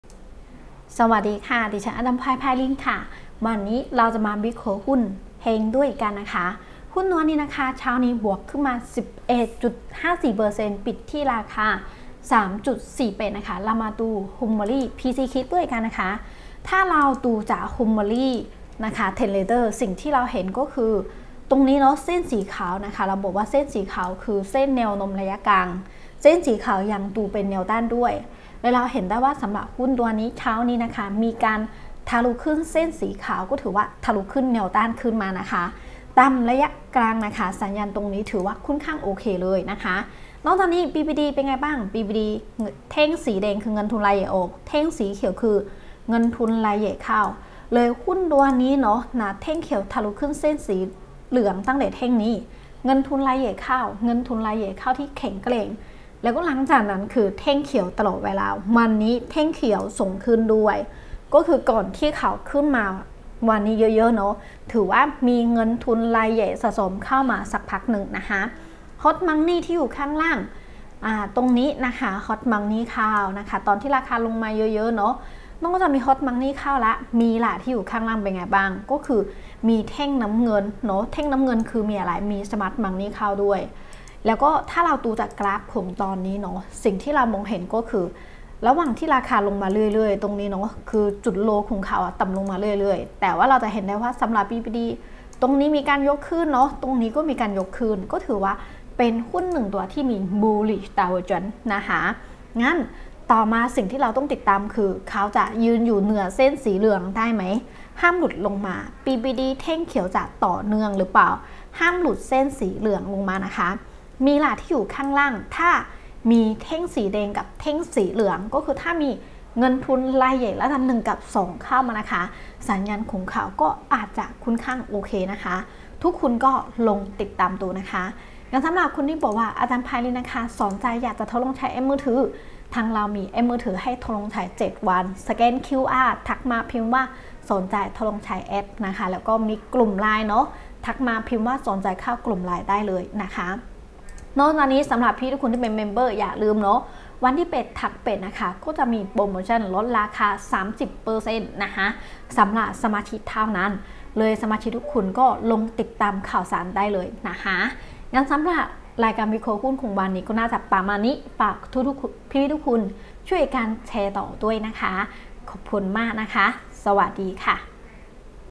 วิเคราะห์หุ้น HENG ด้วยกราฟhomilY chartกับอาจารย์พลอยไพลิน25/07/2022
วิเคราะห์หุ้น HENGด้วยกราฟhomilY chartกับอาจารย์พลอยไพลิน23/07/2022
ช่องมางติดตามวิธีเล่นหุ้นตามวิธีเล่นหุ้นตามอาจารย์ไพลินของhomilychart

ความรู้+เครื่องมือ+วินัย=ความสำเร็จ

ติดตามวิธีเล่นหุ้นตามอาจารย์ไพลิน ด้วย homilychart